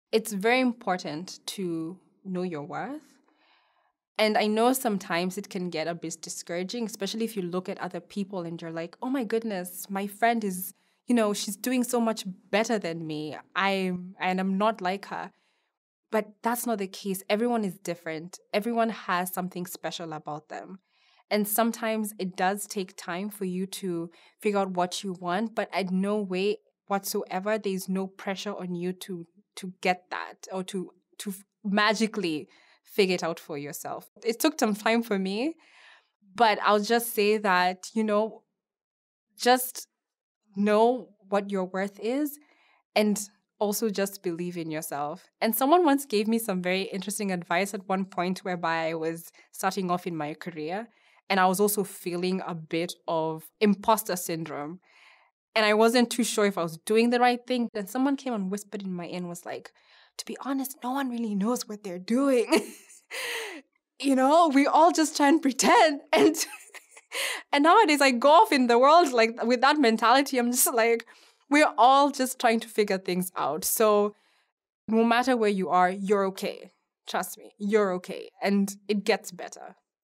Advice | Design Researcher Marian Muthui | Career Girls
Watch Marian Muthui, a graduate student at the Massachusetts Institute of Technology (MIT). Her research focuses on creating a programming language that helps young people get started in coding.